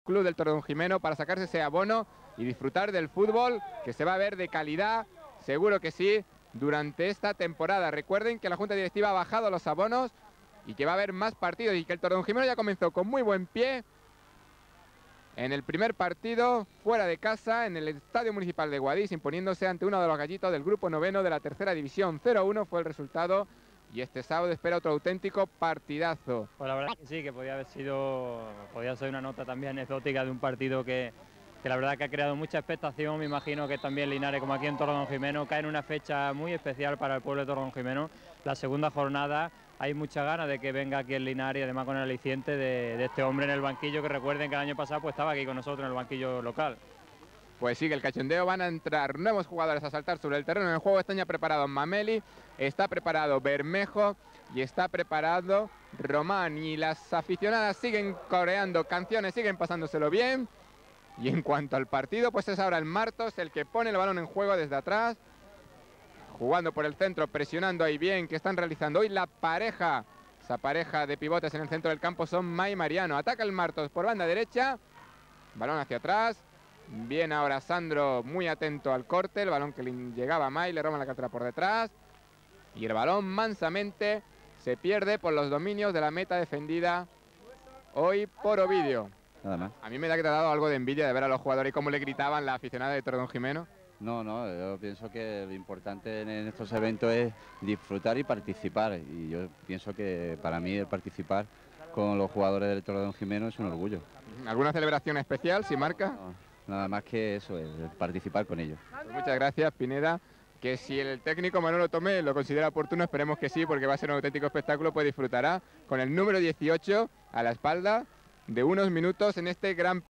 Torredonjimneo c.f.
Gran Premio de Escocia  de futbol en faldas . Dedicado a las mujeres